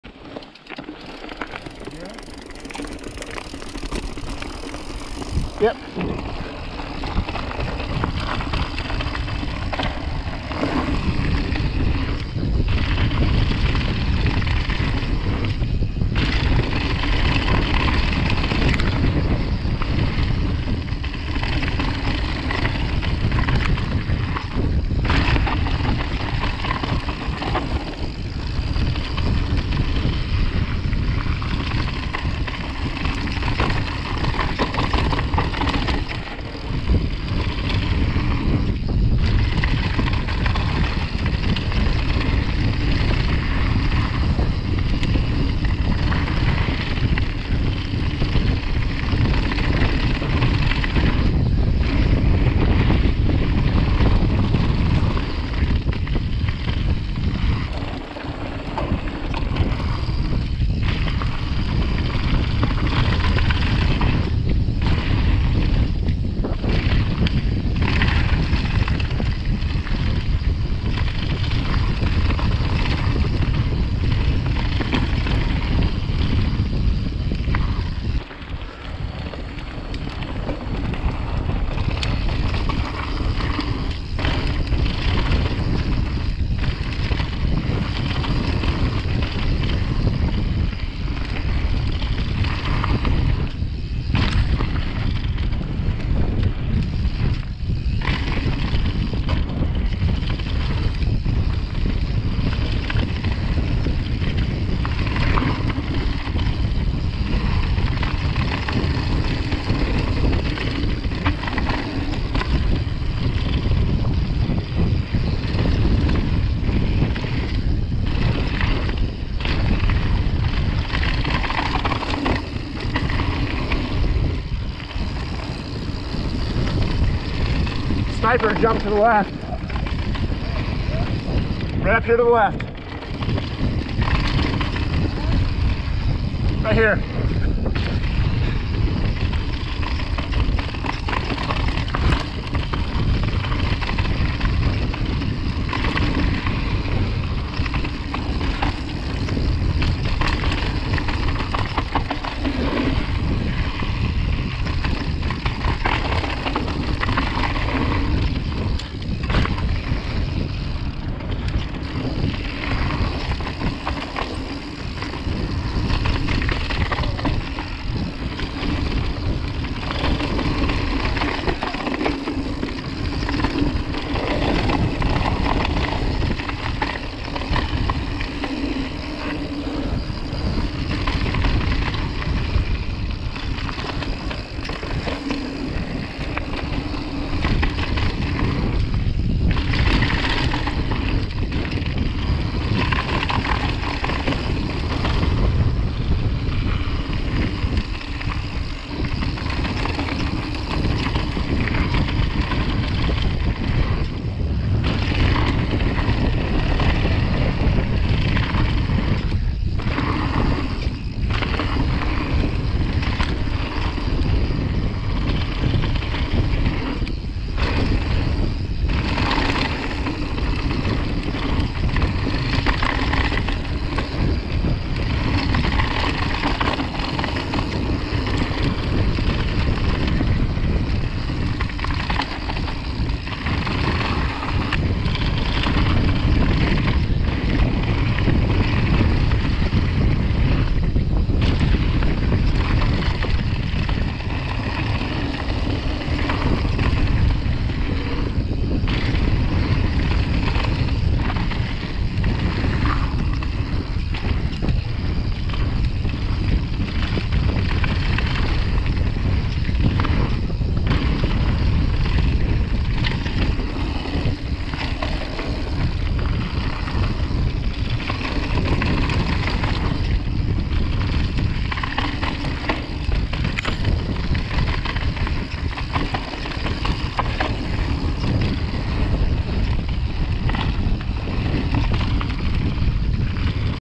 Kashmir Whitefish Bike Park. Whitefish, Montana MTB
Raw follow cam footage of the middle and lower portion of Kashmir at Whitefish Bike Park in Whitefish, Montana.